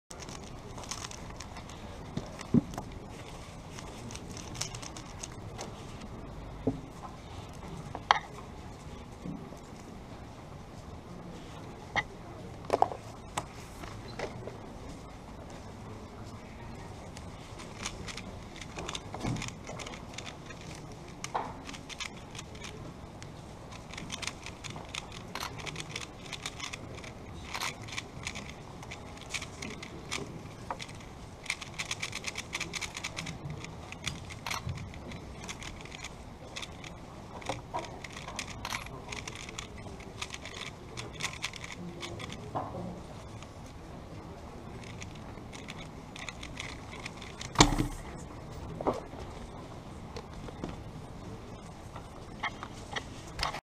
45.60 3x3 Blindfolded official PB!
done in the 3x3 blindfolded final of world championship 2015 in Sao Paulo, Brazil  :)